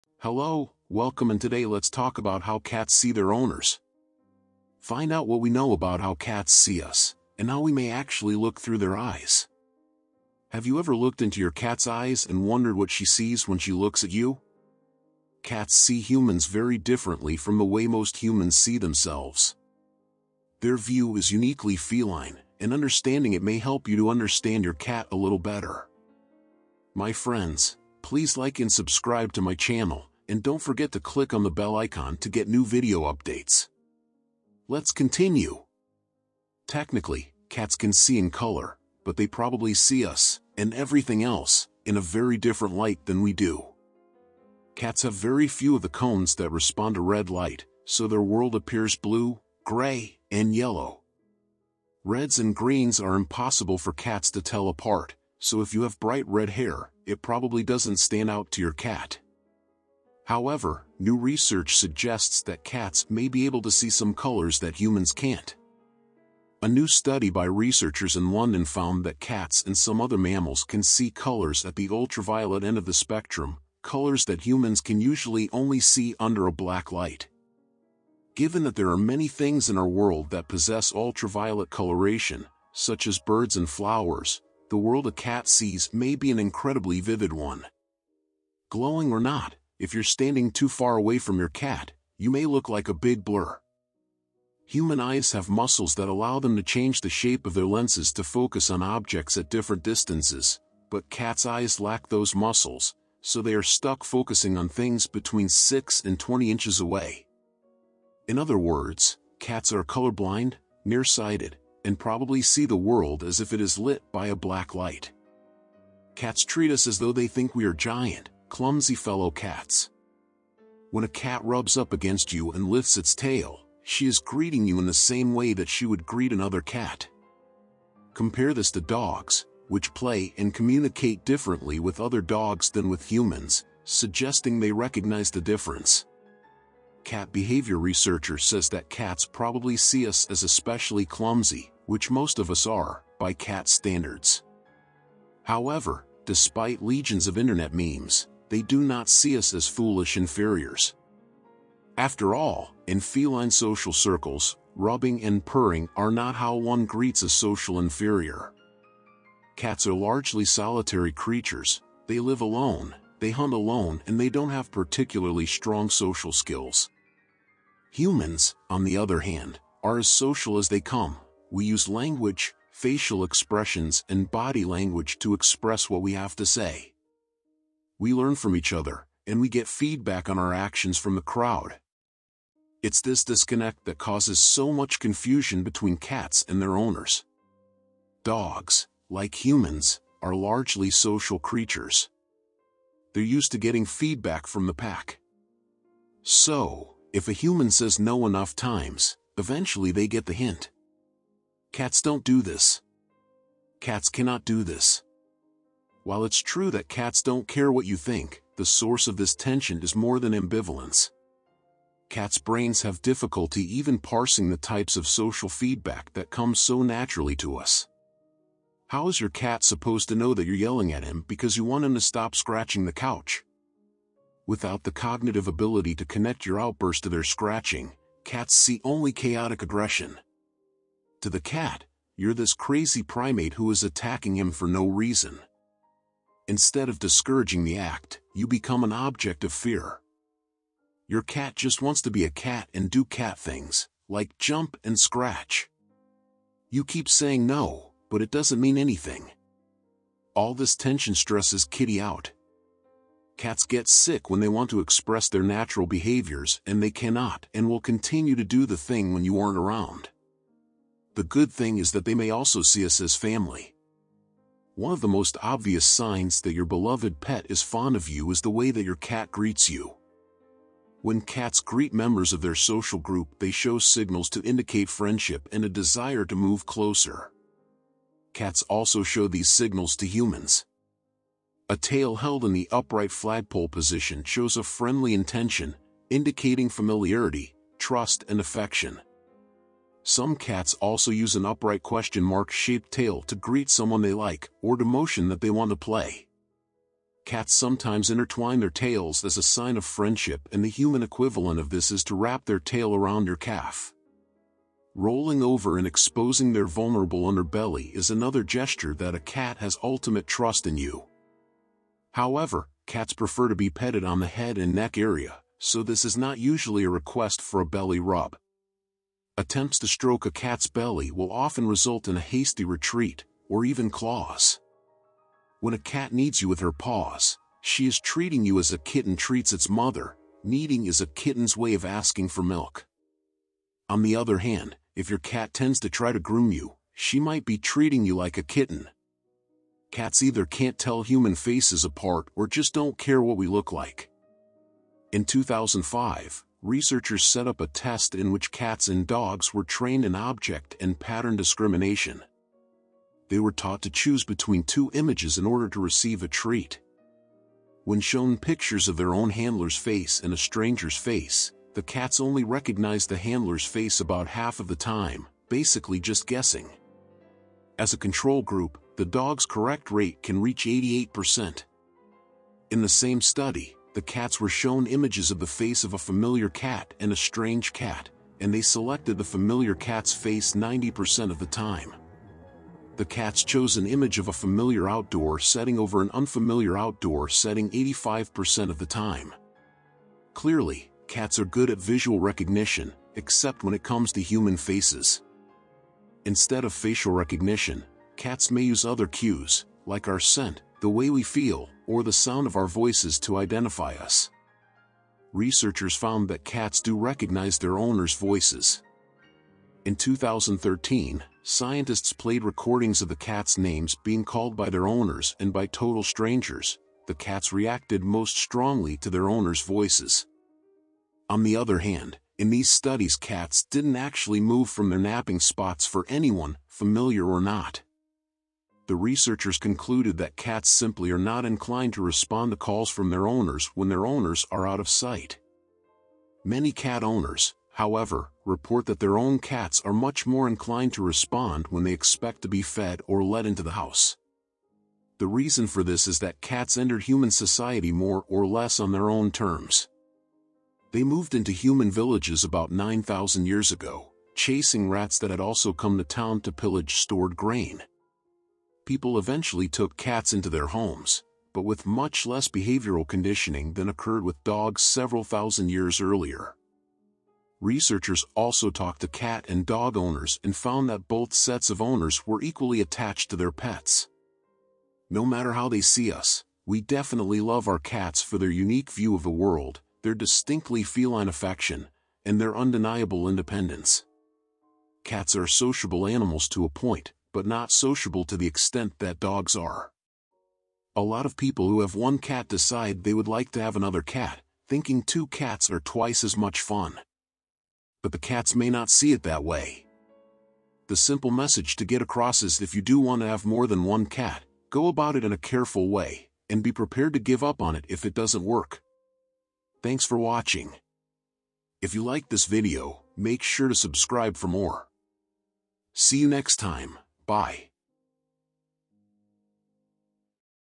Unveiling the Feline Perspective: How Cats See Humans and the World Around Them
Have you ever wondered how cats see the world and humans? Do they see us the same way we see them? Are they colorblind and nearsighted?

In this video, we will explore the fascinating world of feline vision and learn what cats see when they look at us. From their unique visual abilities to their perception of motion, we will uncover the secrets of how cats see the world around them.

So, if you're a cat lover or just curious about these fascinating creatures, join us on this journey to discover the world through a cat's eyes.